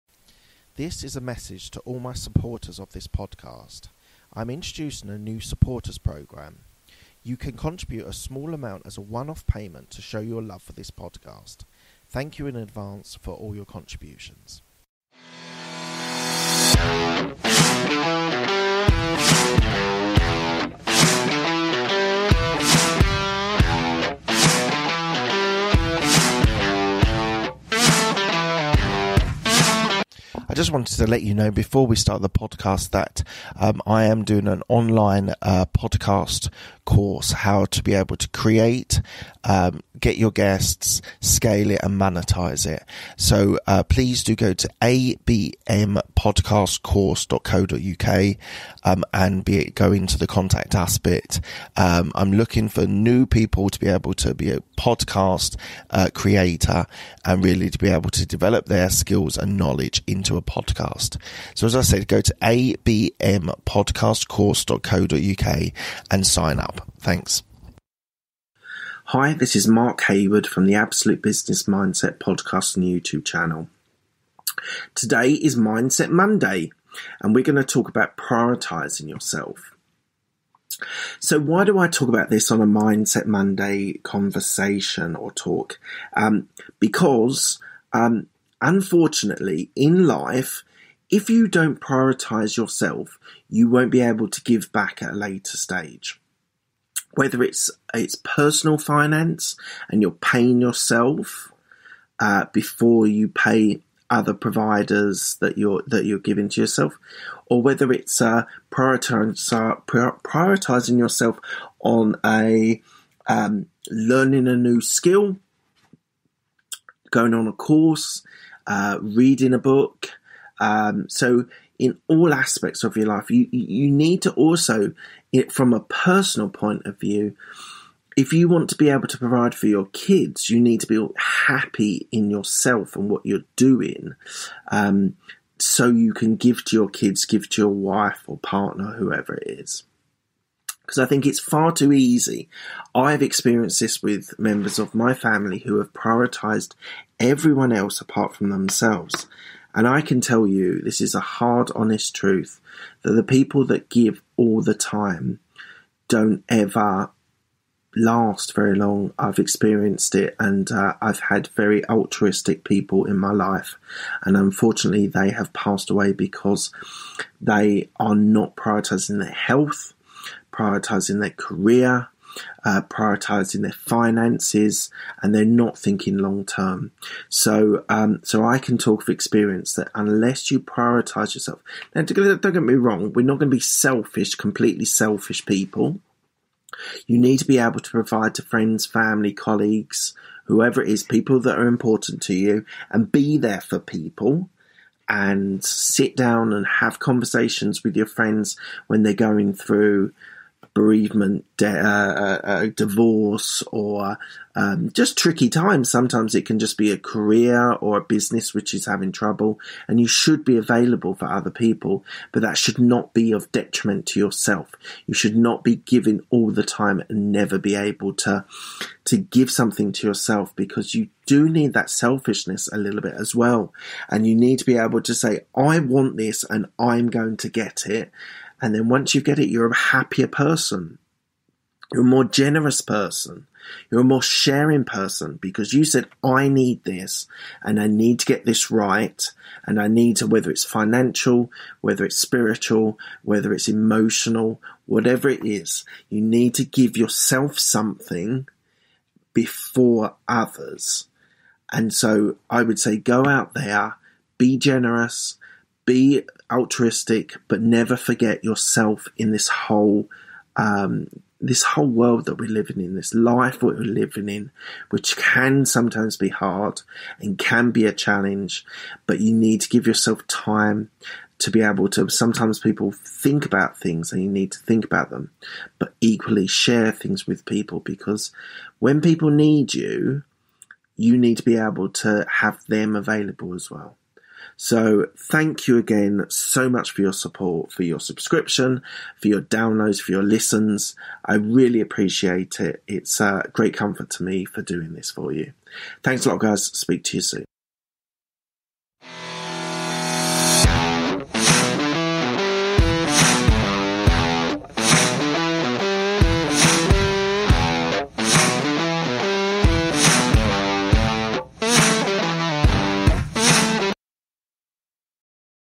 MINDSET MONDAY - Prioritise yourself
I am a corporate employee with an entrepreneurial mindset.

This is Mindset Monday and I am sharing my belief that you should prioritise yourself when running a business. This is not in a selfish way but it is important that you care about yourself to be successful.

Podcast course

for privacy and opt-out information.